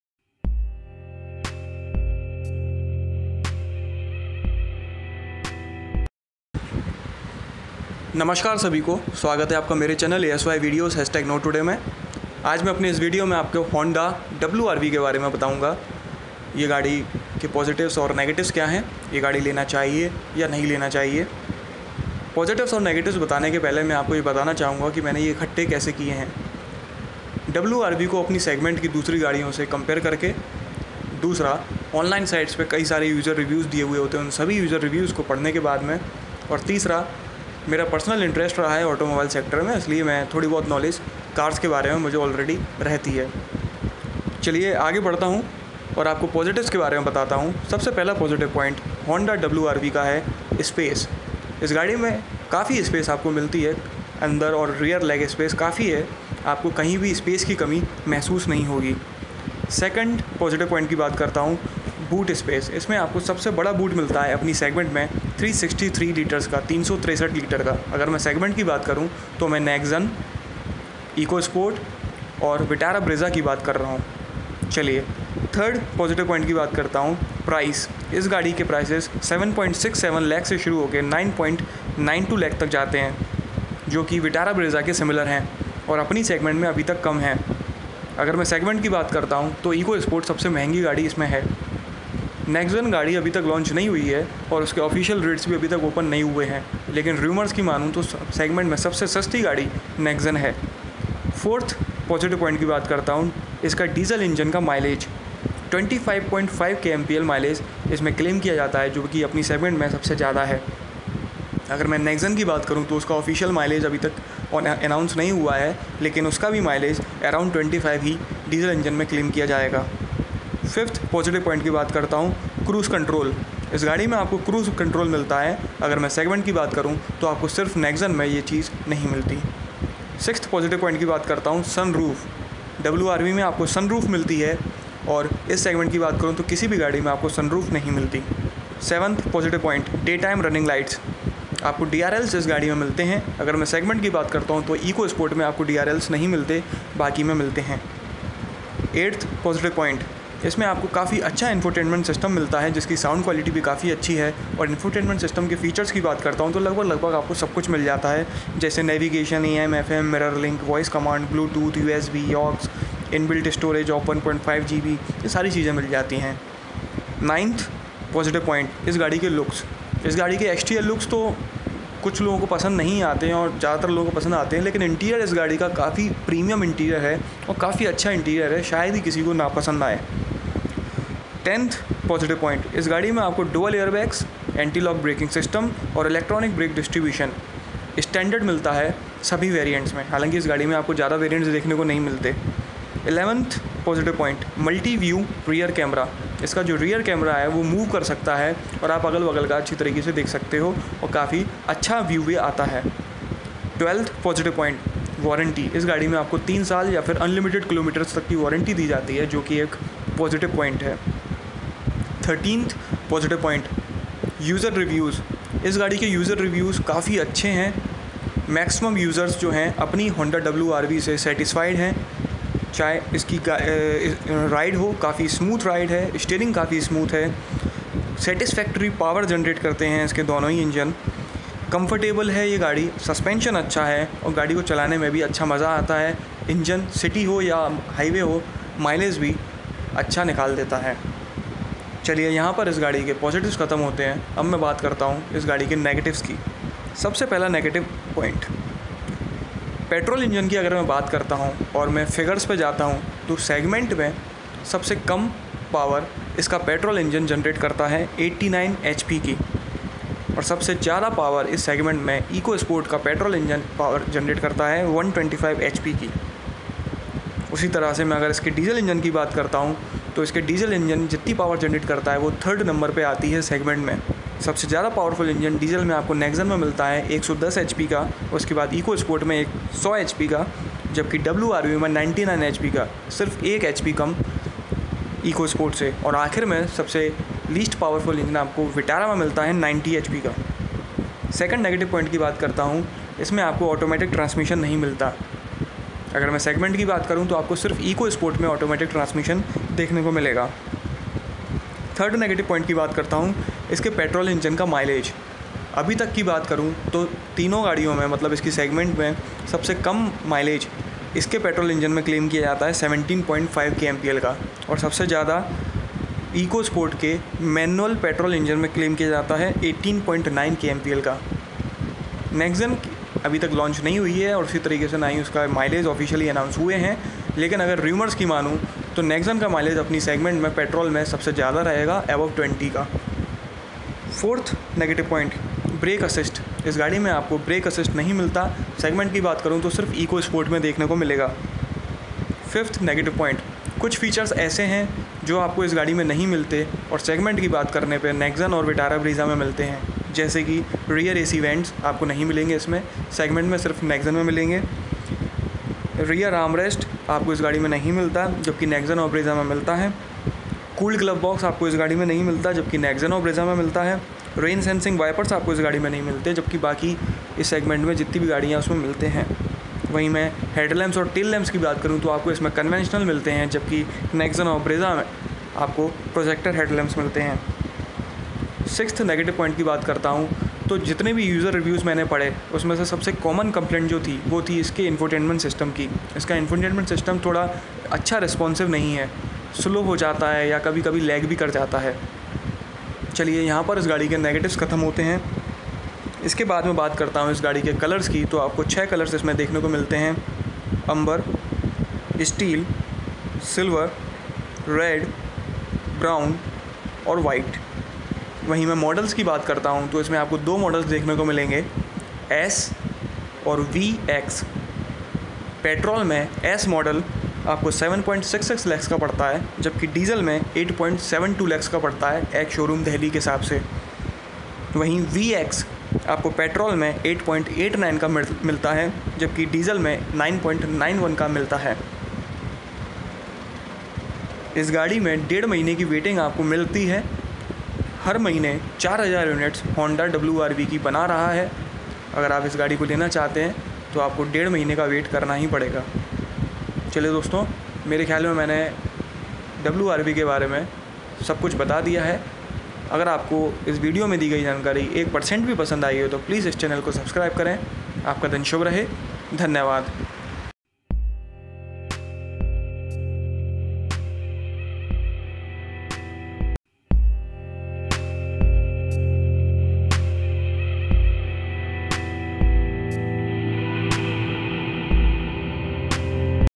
HONDA WR V | WRV REVIEW | WRV 2017 | HONDA W-RV : SHOULD YOU BUY? /COLORS /PRICE
positives & negatives explained in a detailed way which will help you to make right decision for sure.